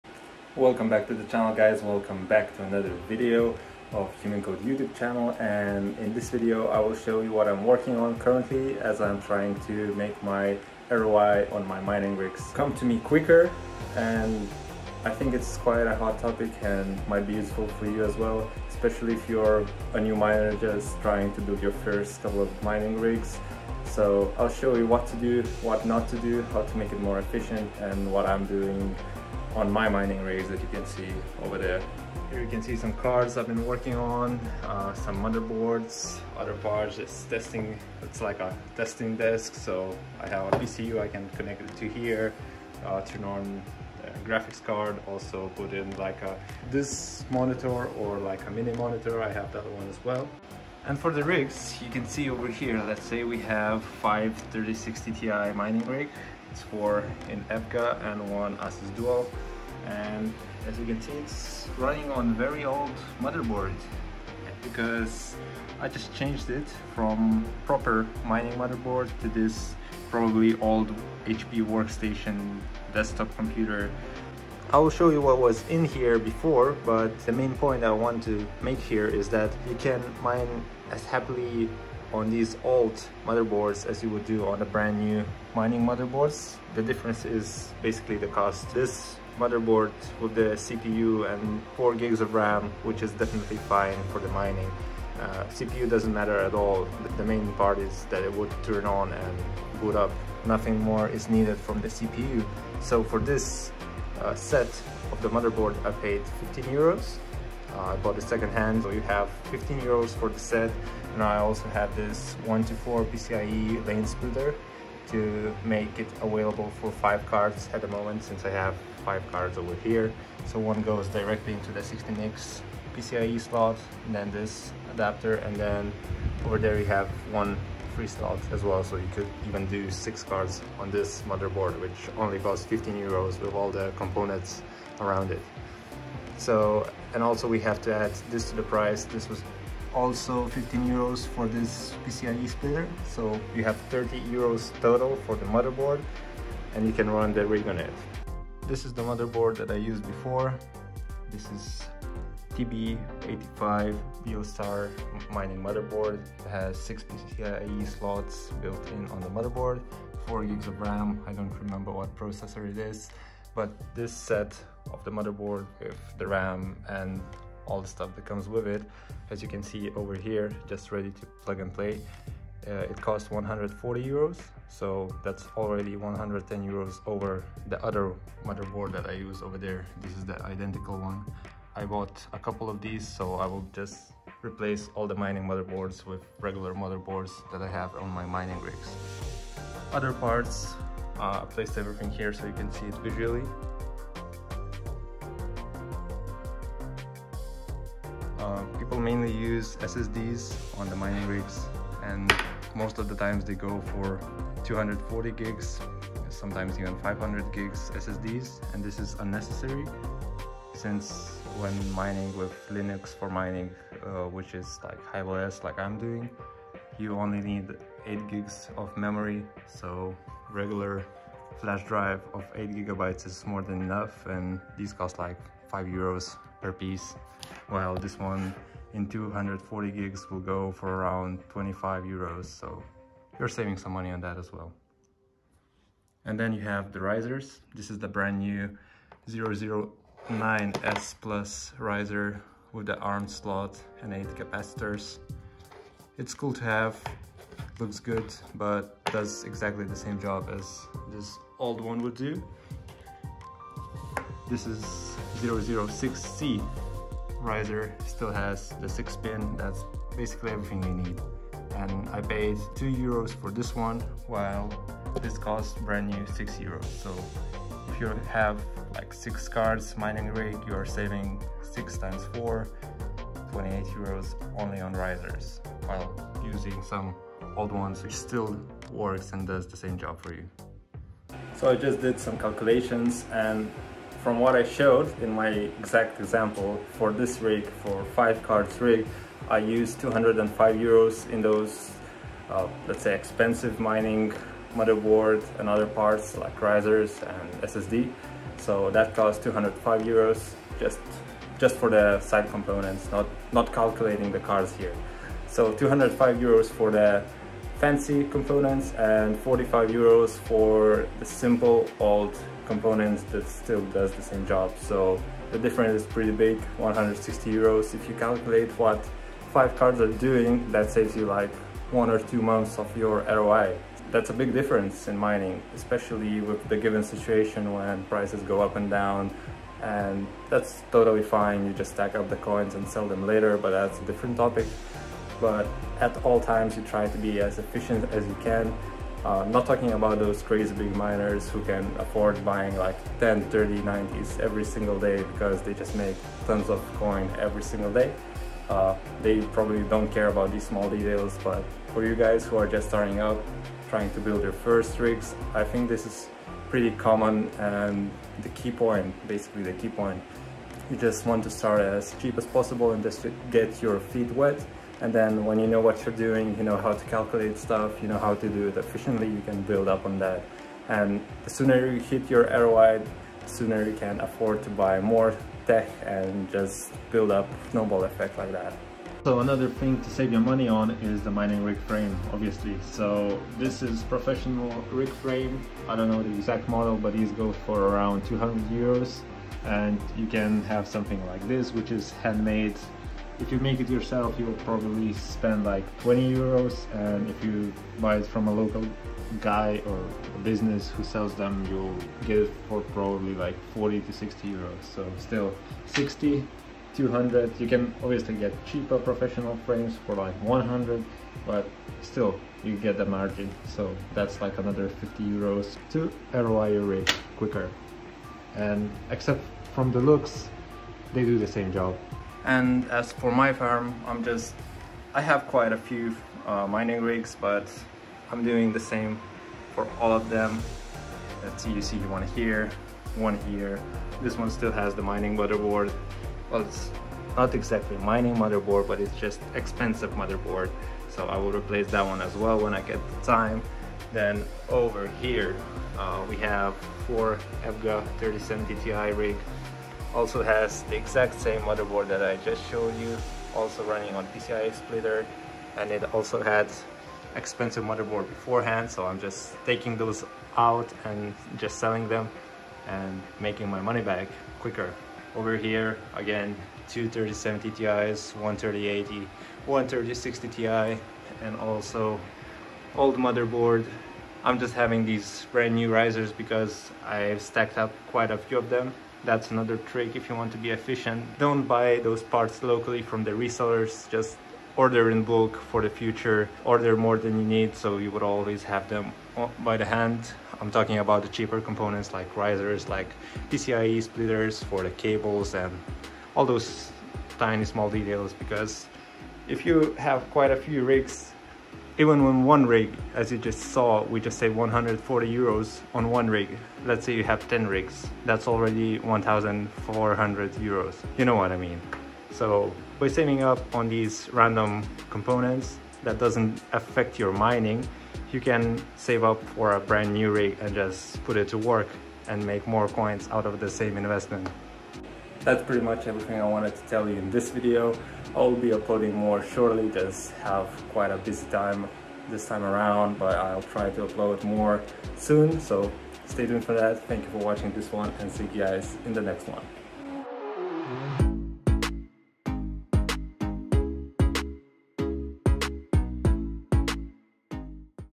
HOW TO BUILD CHEAP MINING RIG - ON BUDGET 2022
Hello there fellow miner!
I will show you how you can build your new Ethereum or any other coin GPU mining rig or optimise existing ones to ROI faster and more cost efficiently.

This will surely help out a freshers, who are trying to get into budget mining, that's exactly how to go from the very start!

Crypto currency mining is still very profitable in 2022 if you do it the right way and I want to make sure that people know what good practices are :)